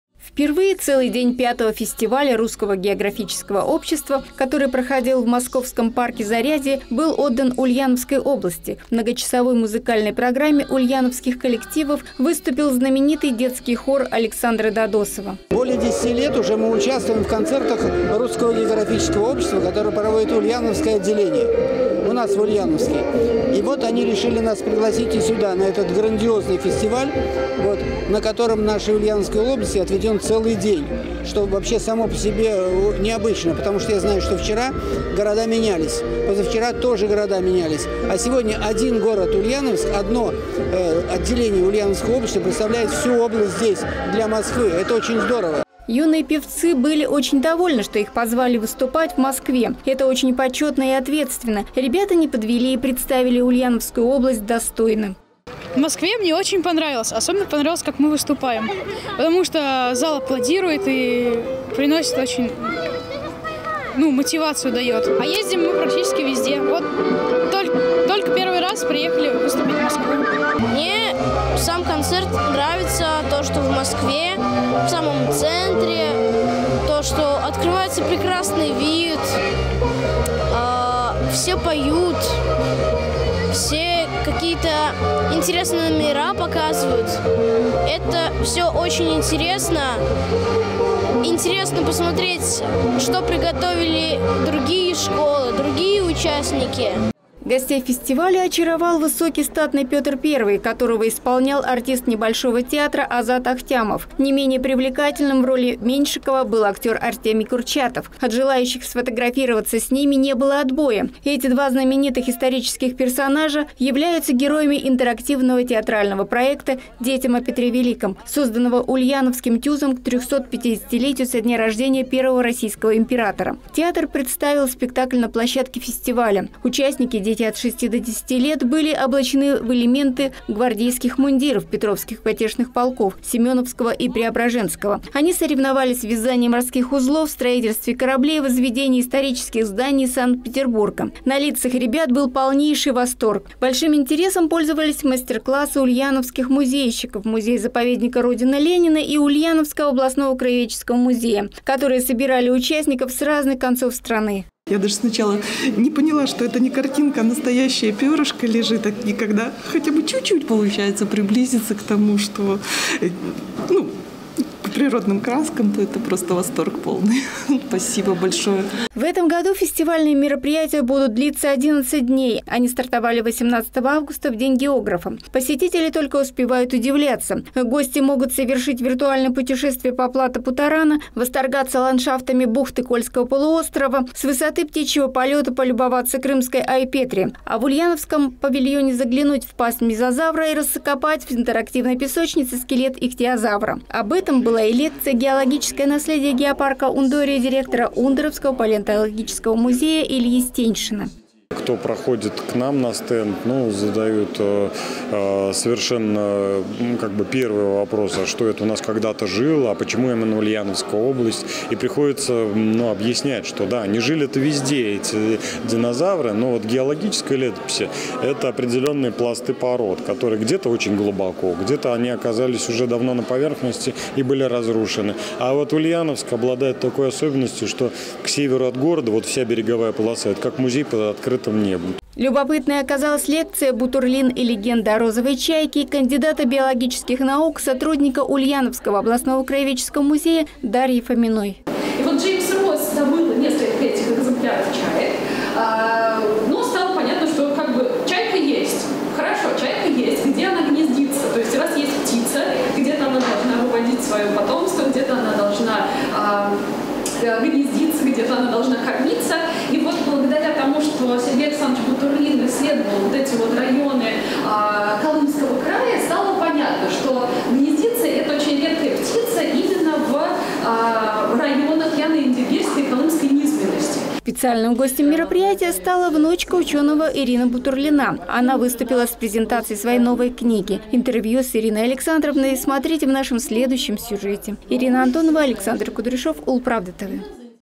Розовая чайка и великий Петр. Чем удивила Ульяновская область на фестивале в Москве
В московском парке «Зарядье» расположился фестиваль Русского географического общества. Помимо официальной части, участники приготовили и яркие презентации своих регионов. Чем удивила гостей фестиваля Ульяновская область - расскажем в нашем материале